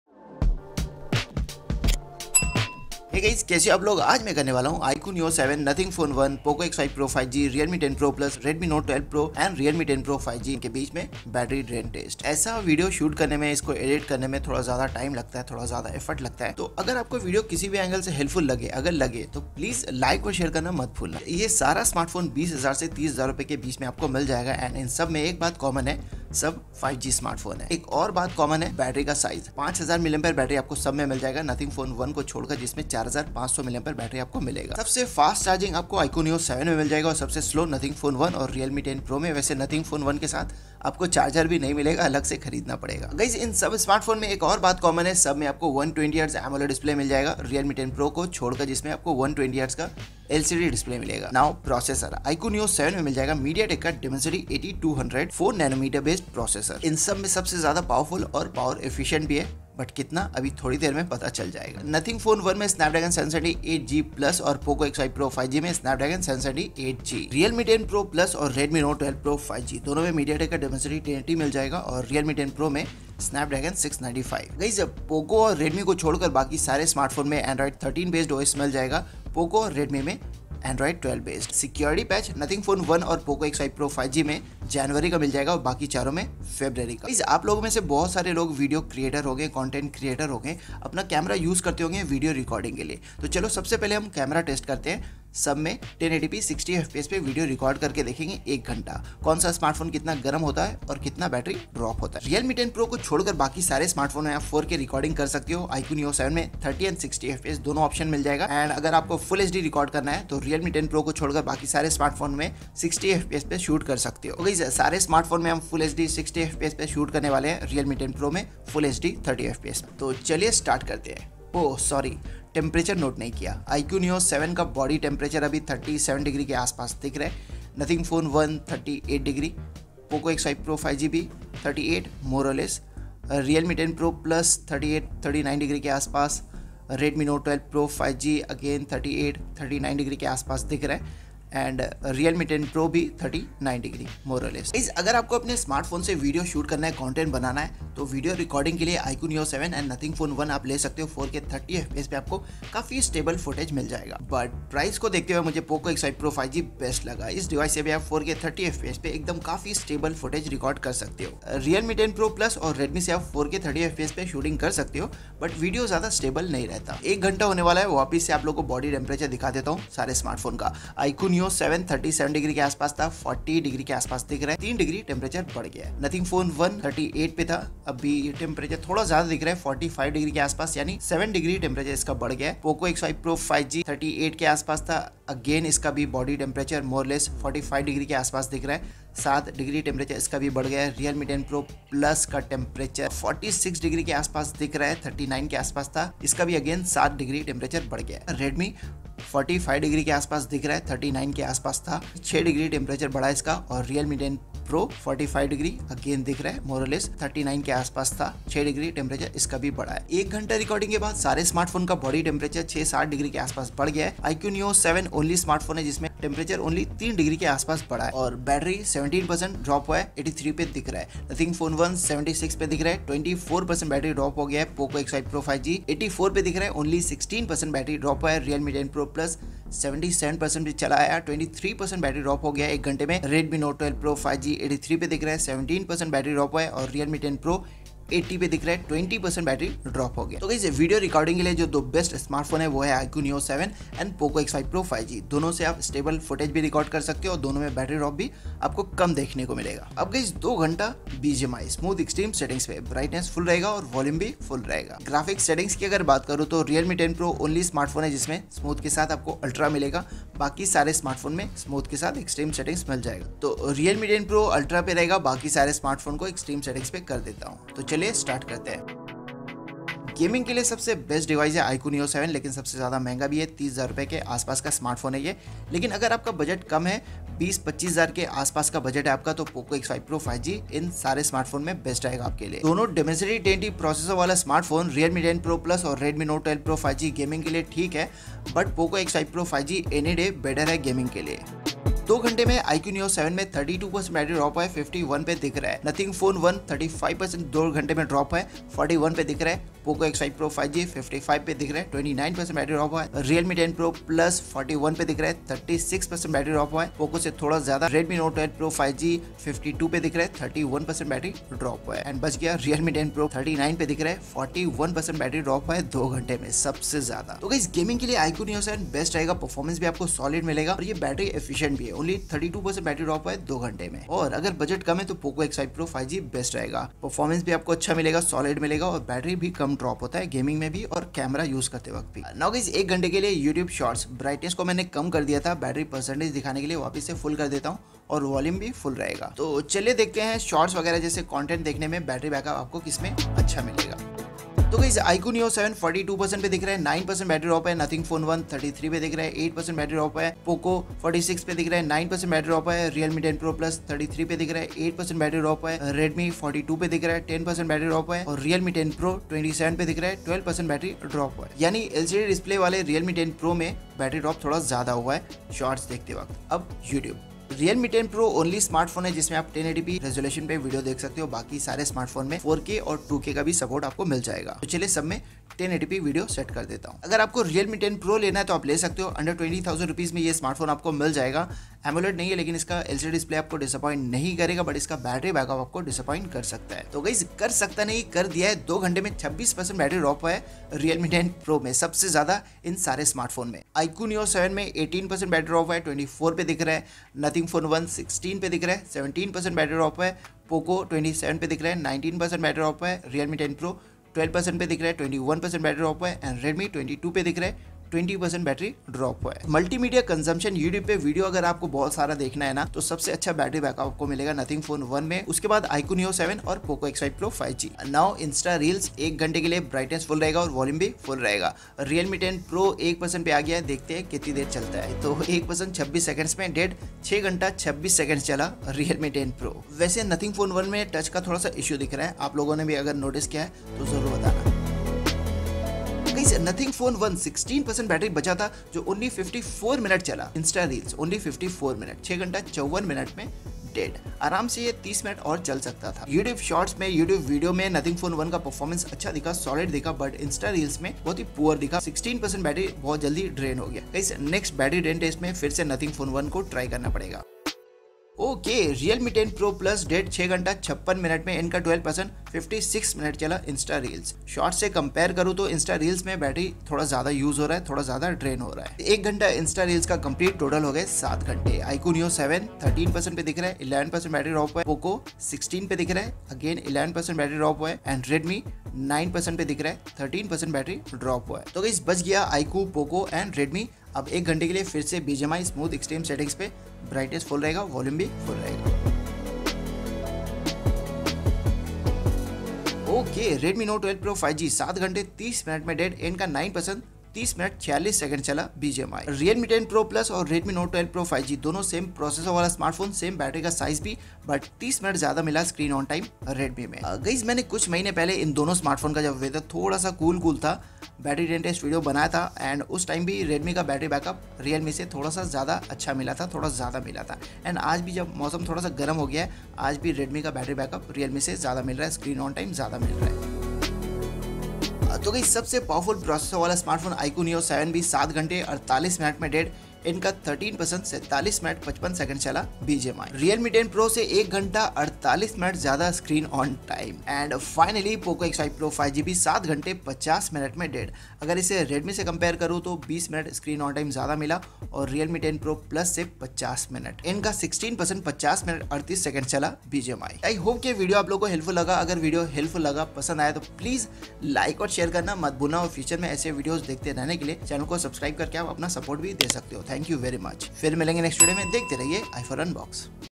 Poco X5 Pro vs iQoo Neo 7 vs Realme 10 Pro+ vs Nothing Phone 1 vs Redmi Note 12 Pro vs Realme 10 Pro
Full Battery Drain and Heating Test - iQOO Neo 7 vs POCO X5 Pro 5G vs Nothing Phone 1 vs Realme 10 Pro Plus vs Redmi Note 12 Pro vs Realme 10 Pro.

Welcome to i4UNBOX, We brings you quality tech videos, unboxing, comparisons, tips and tricks and tech reviews regularly.